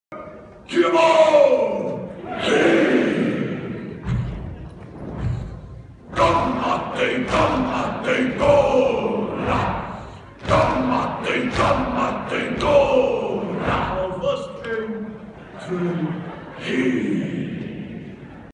Haka tijdens MMV-Congres 2017
Het MMV-congres 2017 (georganiseerd door de Federatie Medisch Specialisten) wordt geopend met een Haka-workshop, die enthousiast meegedaan wordt door de 800 aanwezige opleiders, aios en andere betrokkenen bij de opleiding tot medisch specialist.